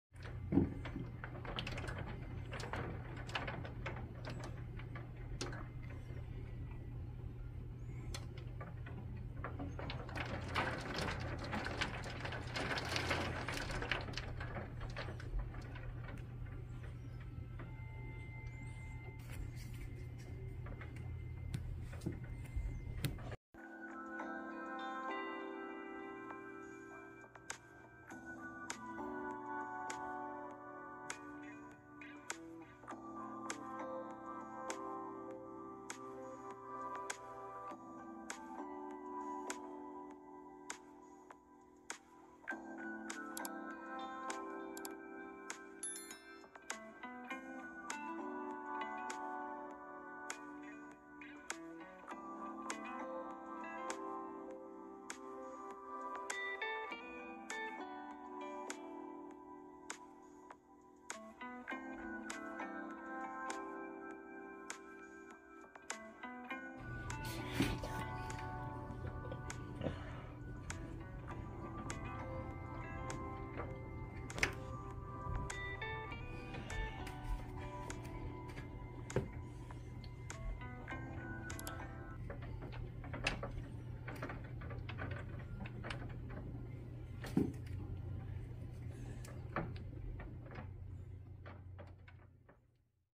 Snowfall and hailing in the mid of April Surrey BC Canada
Weather in middle of April Surrey BC Canada 🇨🇦